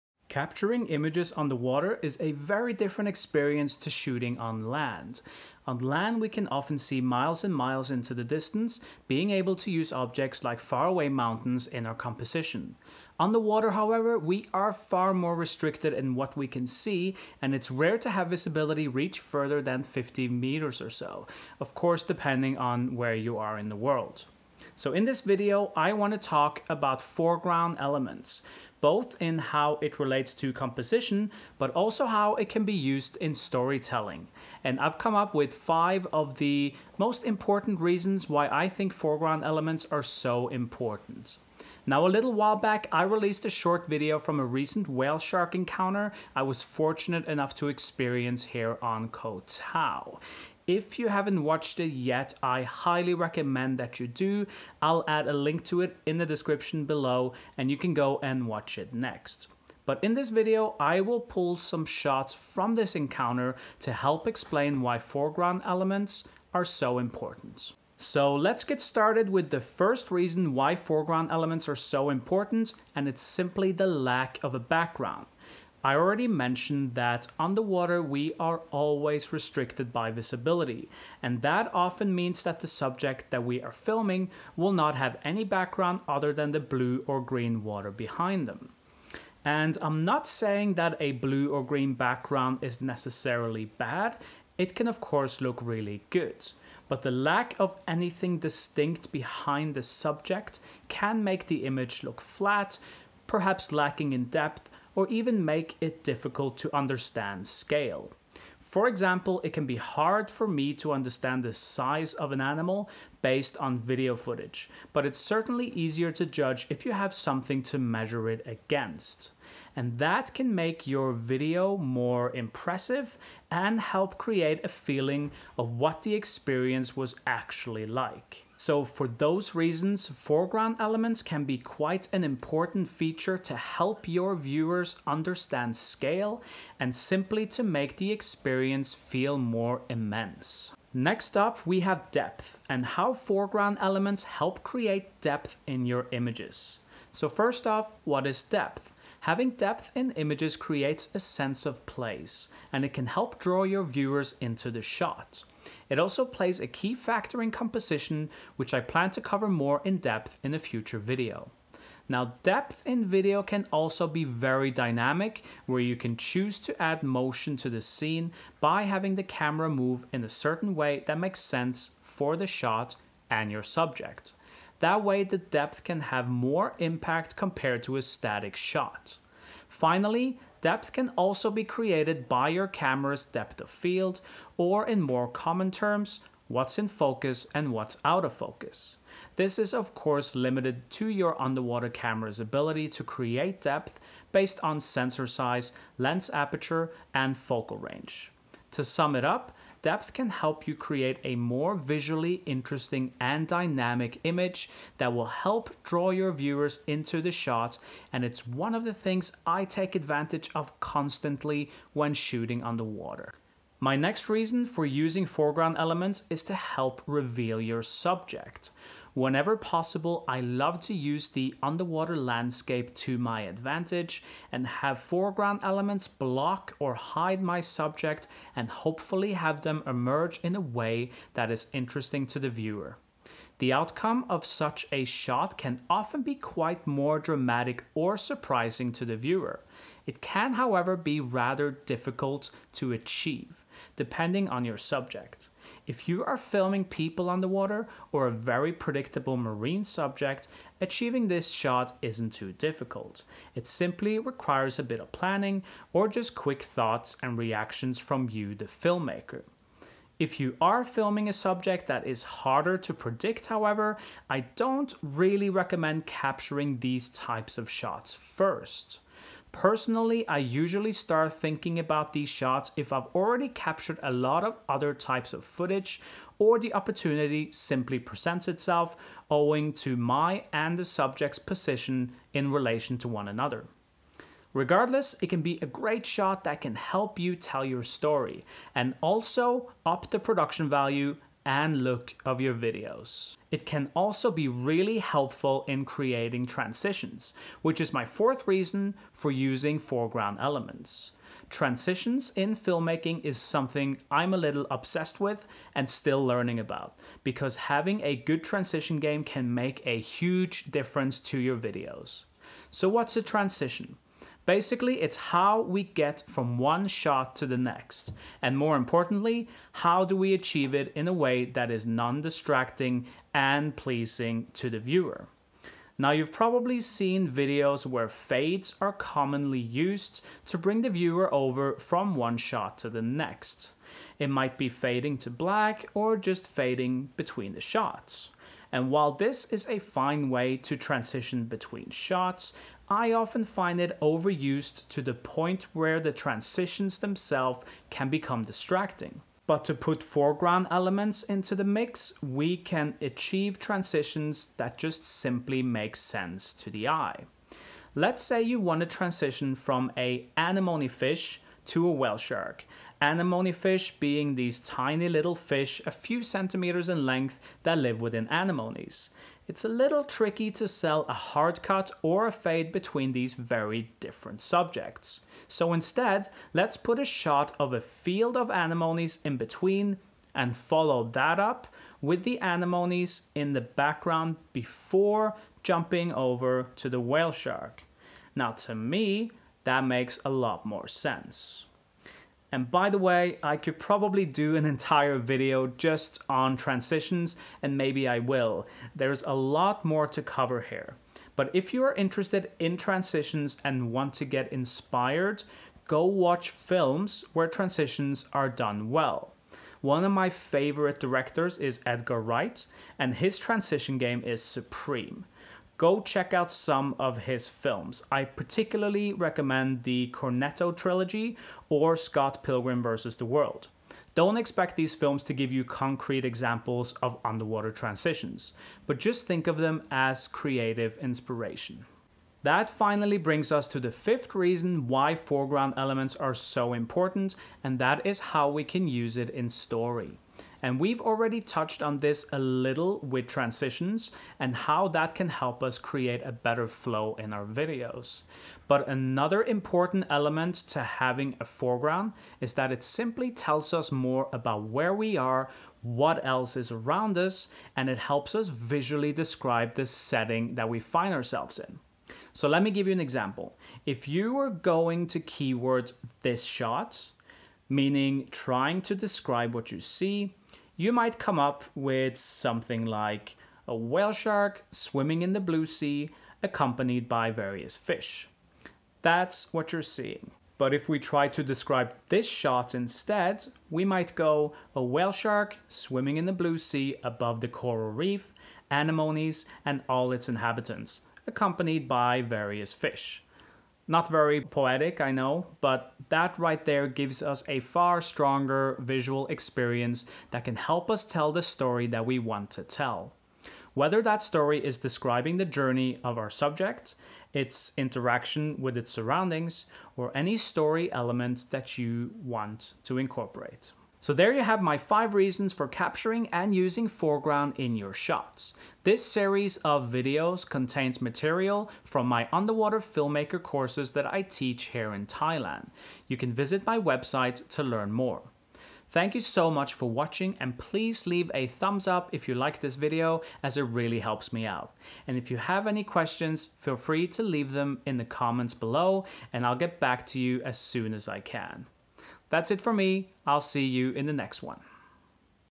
5 Reasons Foreground is Key to Underwater Videography
Making good use of all the tools you have available to capture underwater video is very important, and I've come up with a list of 5 reasons why foreground elements is so essential to use for underwater videography. These are tips that can be utilized almost anywhere and it's a great set of ideas to add to your existing shot types. I hope you enjoy them

CHAPTERS

00:00 Introduction
01:07 #1 Lack of Background
02:21 #2 Depth
03:33 #3 Help Reveal your Subject
04:53 #4 Create Transitions
07:12 #5 Story